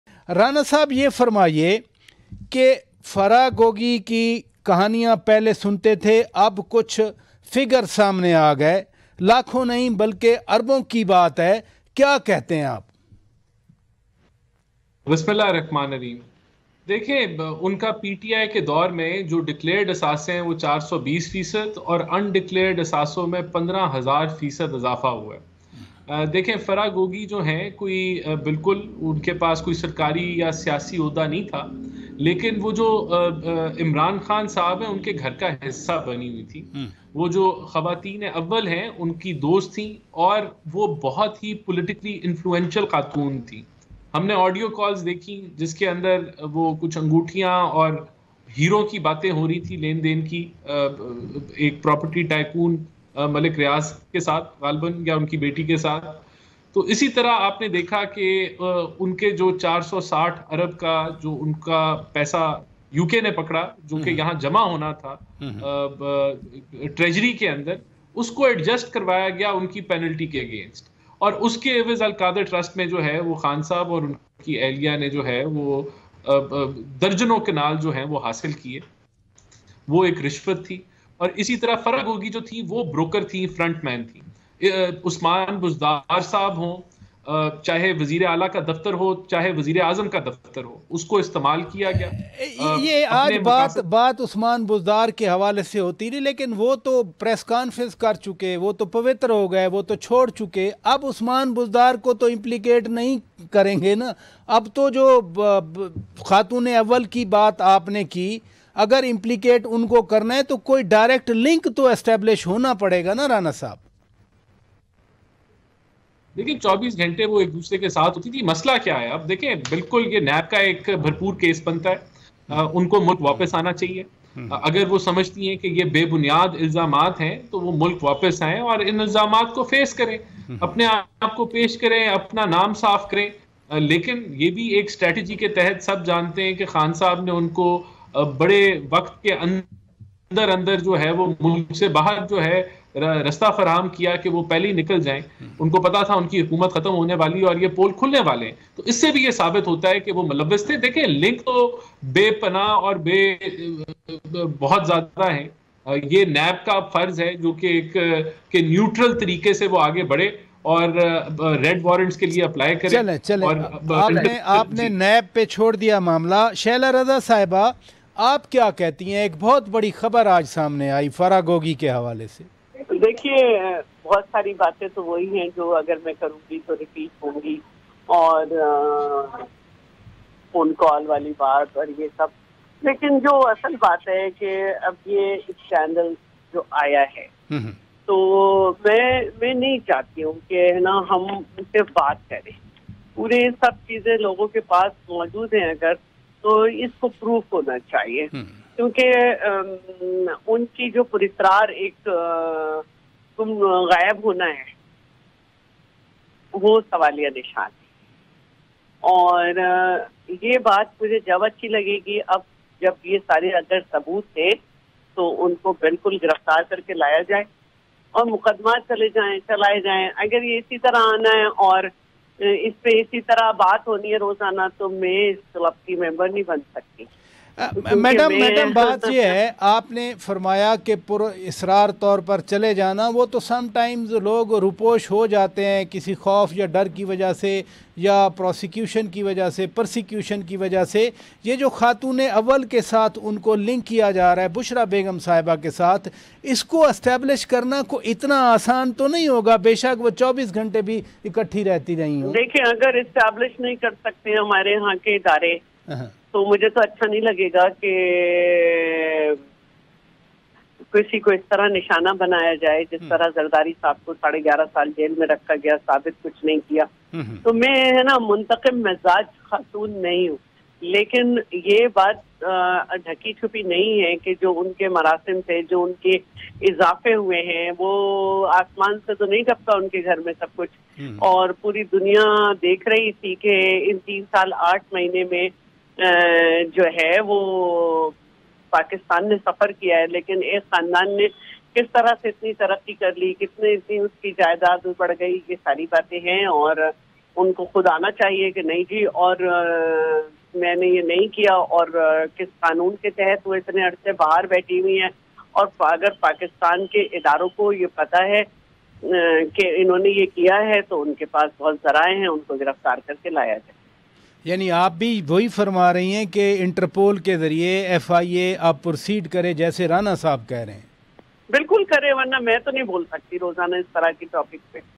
Farah Gogi's assets came out, what will happen now?| Aaj News
Host: Shaukat Piracha

Spot Light With Munizae Jahangir:
shorturl.at/gwFU6